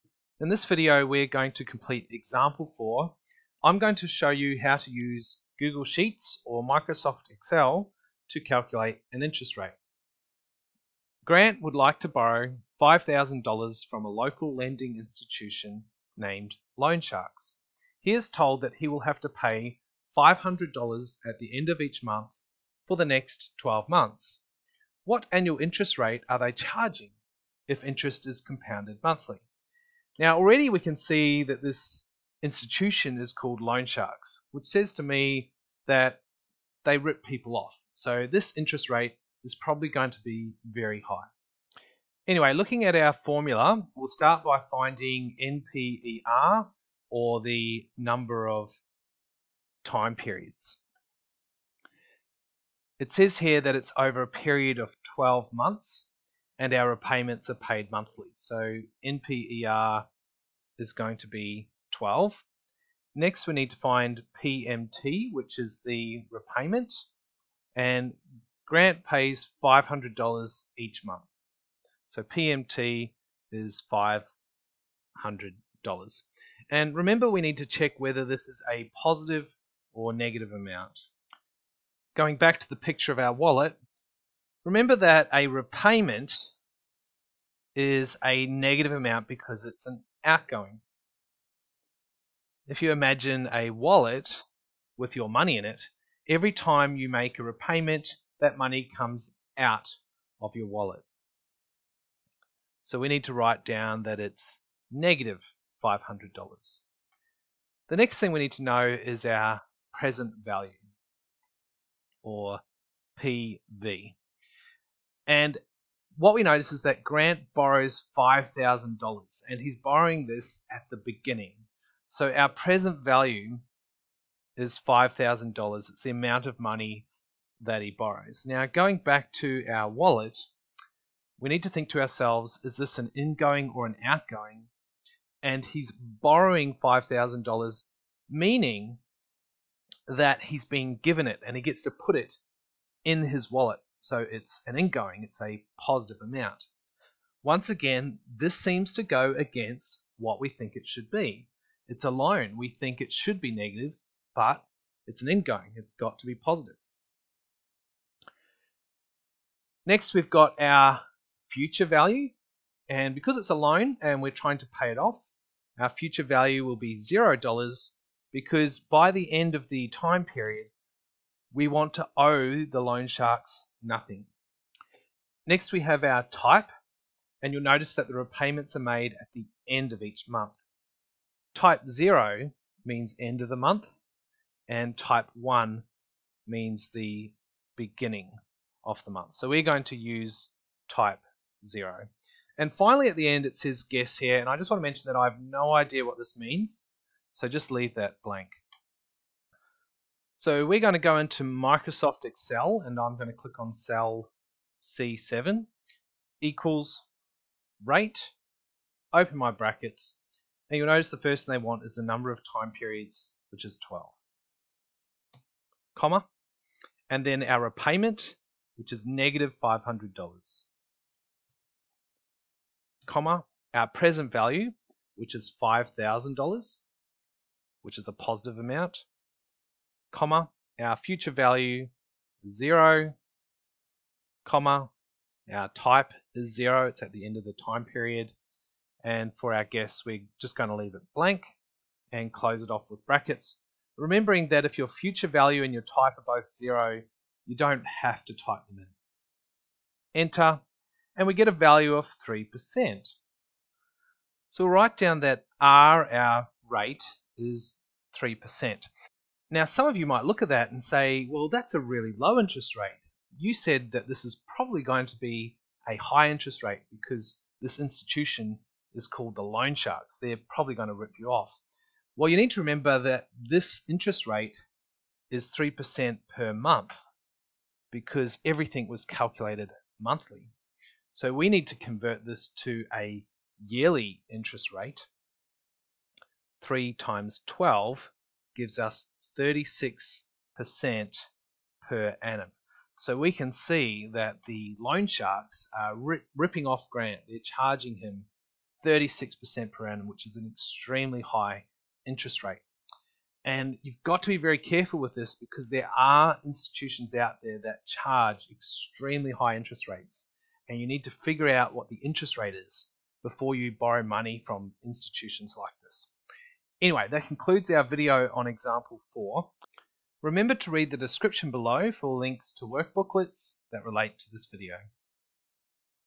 7B Annuities in Google Sheets or Excel (5 of 6)
Explains how to use Microsoft Excel or Google Sheets to calculate the  interest rate of a loan.